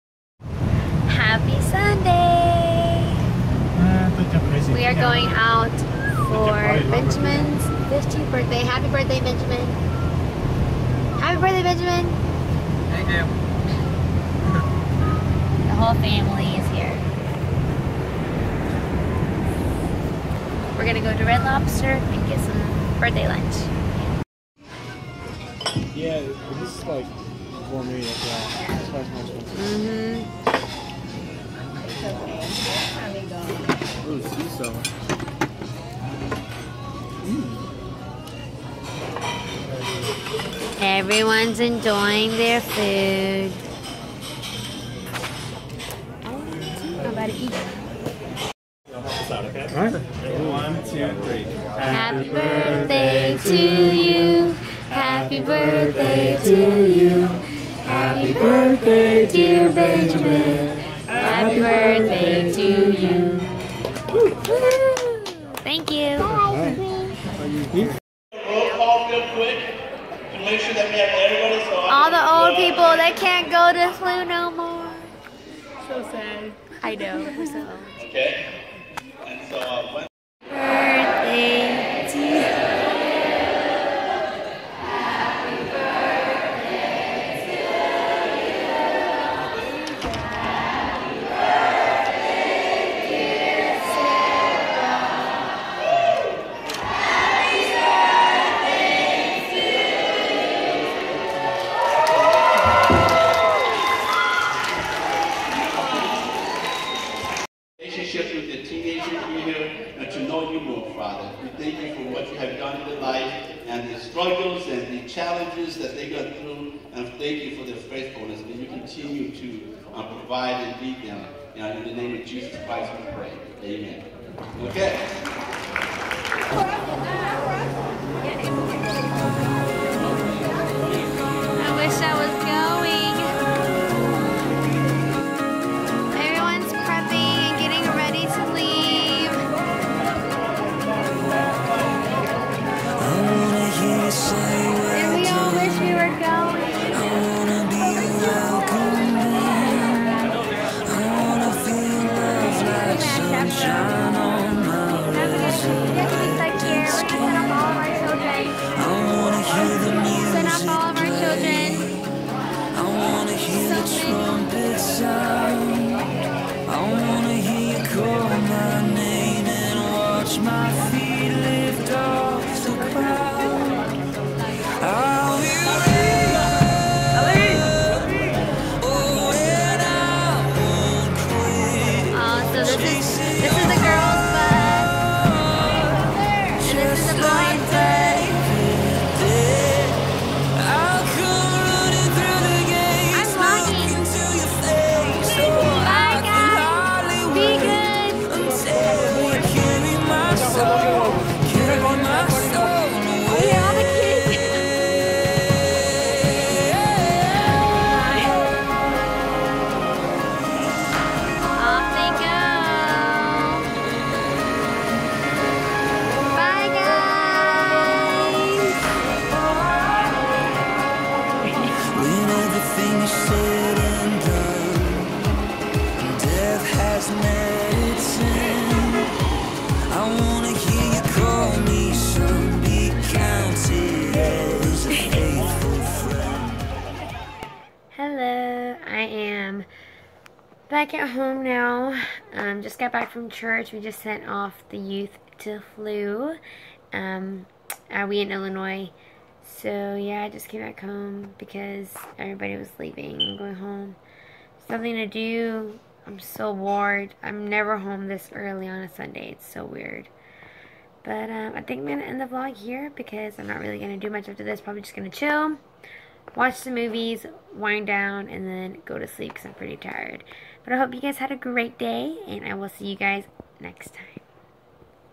Vlog #5: Birthdays & Sending HNHAY off to HLUB!
On this day, July 17th, 15 years ago, my little brother was born into this world. He was the cutest little boy with a head full of hair! Thankful for another year! Praying many blessings over him. Love you, bro. :)

We also sent off our youth group to HLUB camp! So bittersweet sending them off. Wishing I was going with them. :(